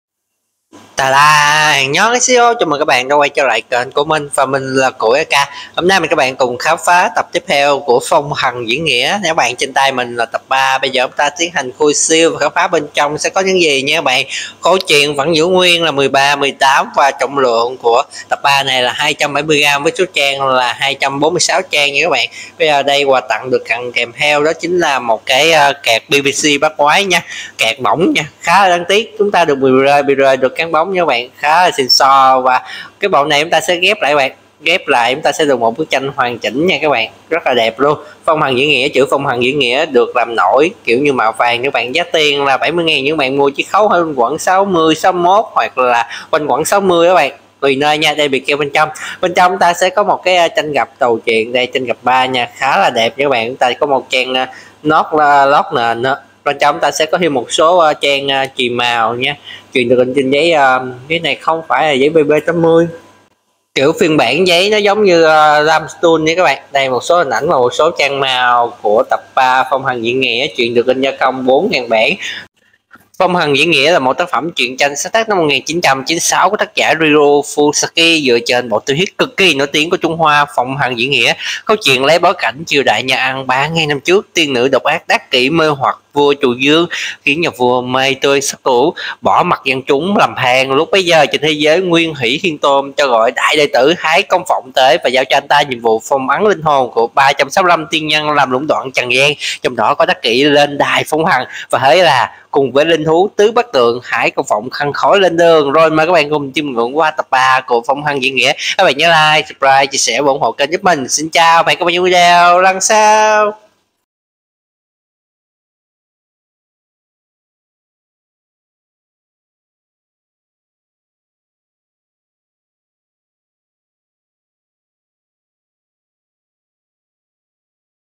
[Review #1467] PHONG THẦN DIỄN NGHĨA TẬP 3 KÈM LÓT LY
Phong Thần Diễn Nghĩa - Tập 3

Một tác phẩm truyện tranh sáng tác vào năm 1996 của tác giả Ryu Fujisaki dựa trên bộ tiểu thuyết cực kì nổi tiếng của Trung Hoa: Phong Thần Diễn Nghĩa! Câu chuyện lấy bối cảnh triều đại nhà Ân 3000 năm trước, tiên nữ độc ác Đát Kỷ mê hoặc vua Trụ Vương, khiến nhà vua đam mê tửu sắc, bỏ mặc dân chúng đói khổ lầm than. Lúc bấy giờ, trên thiên giới, Nguyên Thủy thiên tôn cho gọi đại đệ tử Thái Công Vọng tới và giao cho anh ta nhiệm vụ phong ấn linh hồn của 365 tiên nhân làm lũng đoạn trần gian (trong đó có Đát Kỷ) lên Đài Phong Thần… Và thế là, cùng với linh thú Tứ Bất Tượng, Thái Công Vọng khăn gói lên đường…